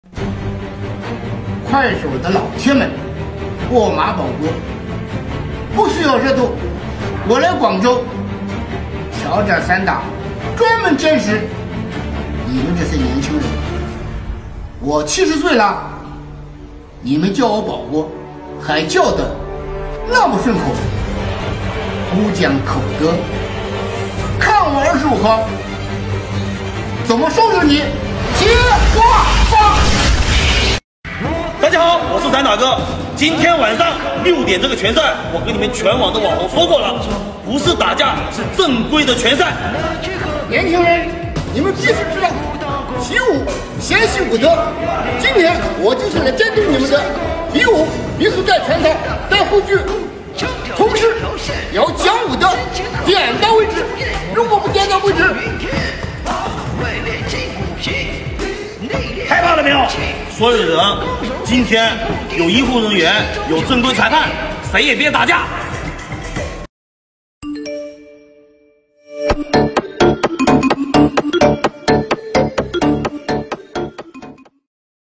马保国快手视频：年轻人不讲口德；监督散打比赛；接化发特效向（纯搬运，来源见视频末尾）
马保国！辣个蓝人！又回来啦！